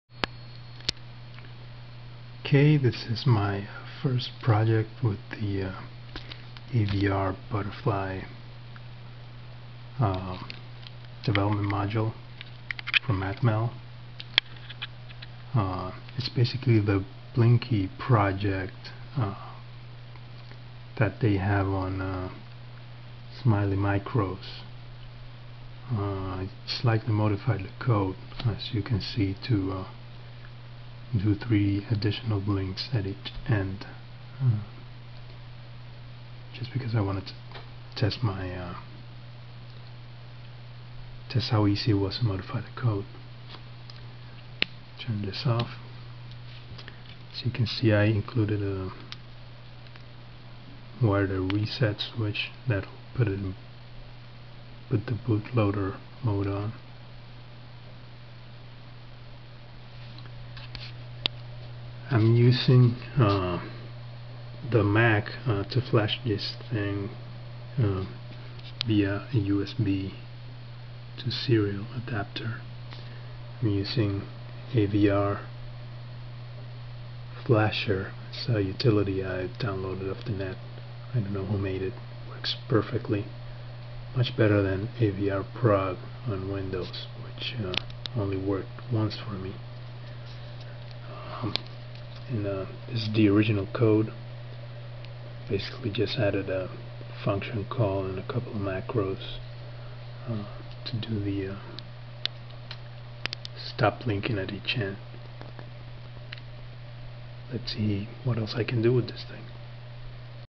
First AVR Butterfly test (Blinky2)
AVR Butterfly Blinky project slightly modified from the version on C PROGRAMMING FOR MICROCONTROLLERS (available